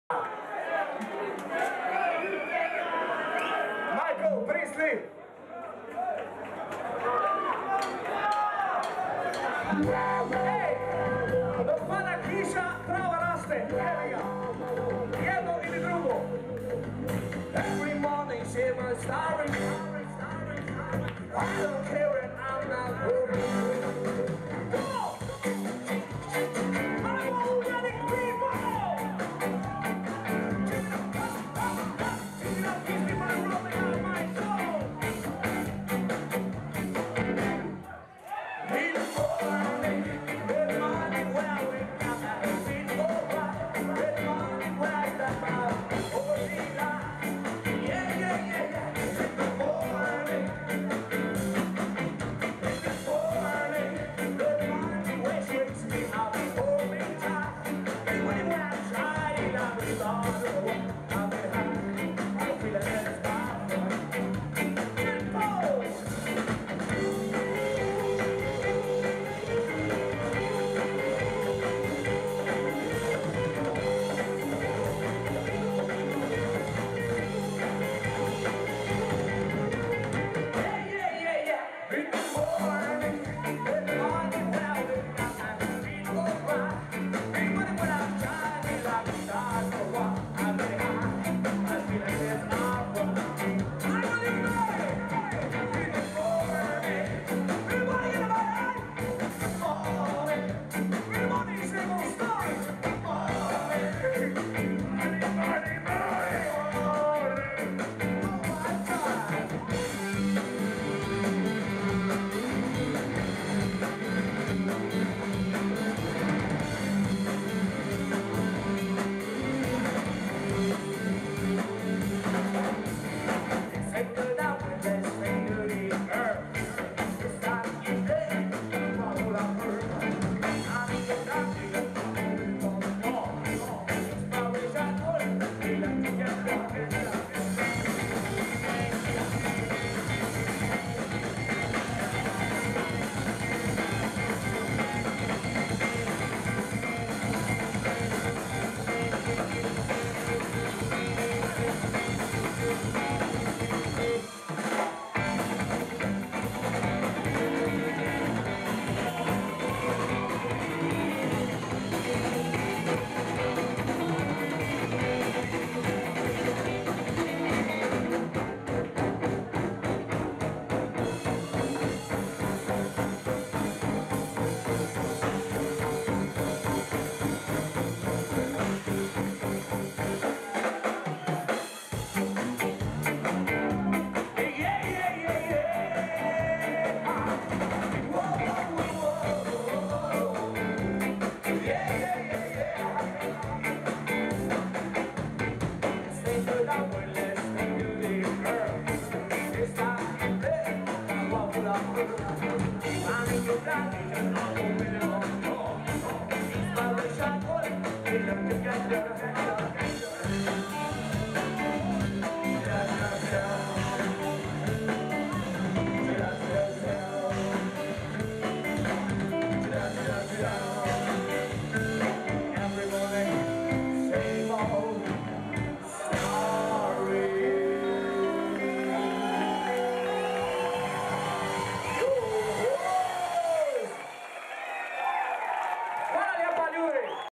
Viva La Pola! 2005 - ELVIS JACKSON (4)
Band from Nova Gorica, Slovenia. Elvis Jackson are an altogether special rock group that has developed its own musical expression over a decade through the help of ska, punk, hard core, reggae and metal influences (Bad Brains, Slayer, Sublime, Beatsteaks, RKL among others). From a band was formed at the end of 1997 and had the first practices two days prior to their first concert, it has evolved into a tight unit of music veterans, famous for their live concerts and positive energy.